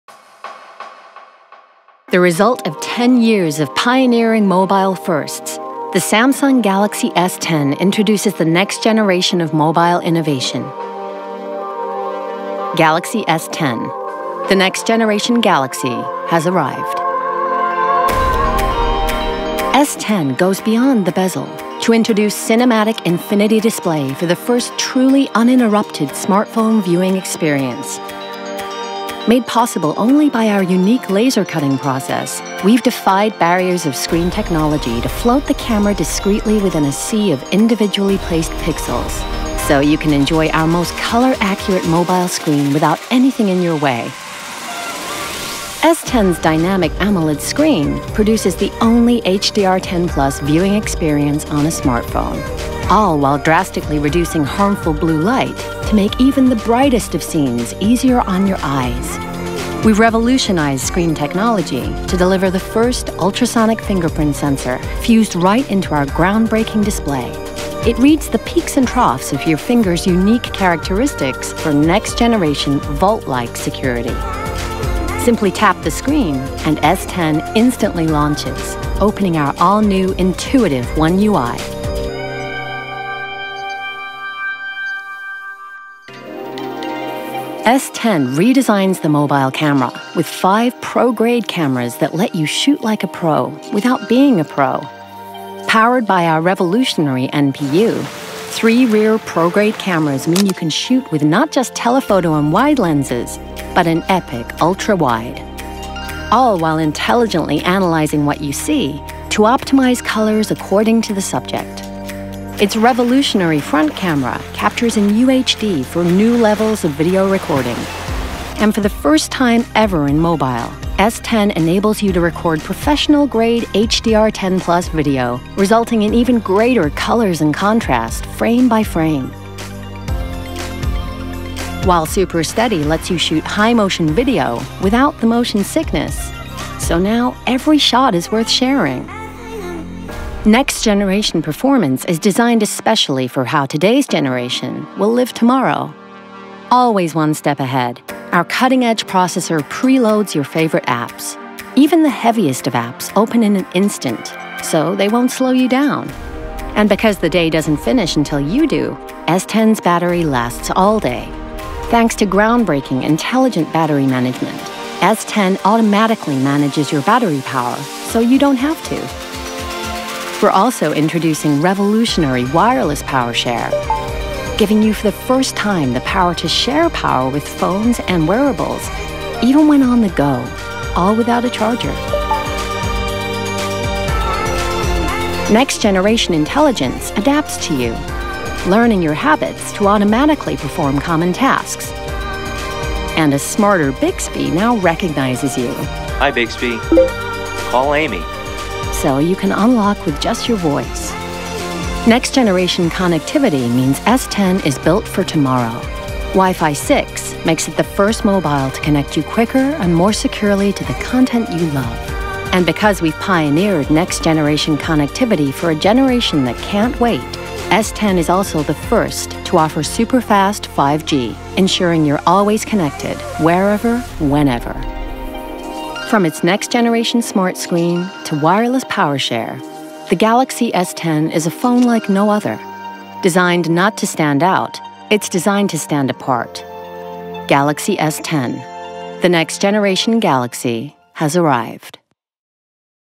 Galaxy S10: Official Introduction
The GalaxyS10 is a next generation smartphone like no other. Designed not to stand out, it’s designed to stand apart.

[Next Generation Screen]
Galaxy S10 defies the barriers of screen technology with its Cinematic Infinity Display. It covers the full front of the phone - from top to bottom and from side to side, giving you the first truly uninterrupted HDR10+ viewing experience.

[Next Generation Security]
The future of smartphone security is fused right into S10’s display. Ultrasonic technology and machine learning work together to instantly capture the unique 3D characteristics of your fingerprint, for vault-like security only you can unlock.

[Next Generation Pro-grade Camera System]
With Galaxy S10, you can shoot like a pro without being a pro. Its next generation pro-grade camera system is powered by an advanced NPU that intelligently analyzes and optimizes what you shoot, and features telephoto, wide, and ultra-wide lenses. Front cameras now capture in UHD, and for the first time ever in a mobile device, you can record professional-grade HDR10+ video.

[Next Generation Performance]
S10’s next generation performance is designed especially for how today’s generation will live tomorrow. Intelligent Battery Management automatically manages your battery power with highly advanced smart solutions, and Wireless PowerShare revolutionizes how you charge your devices.

[Next Generation Connectivity]
Galaxy S10 introduces the future of connectivity with next generation Wi-Fi 6 and 5G. Connect to faster, more secure networks with Wi-Fi 6, while the Galaxy S10 5G also delivers super-fast download speeds at up to 10 gigabits per second (Gbps).
The next generation Galaxy has arrived.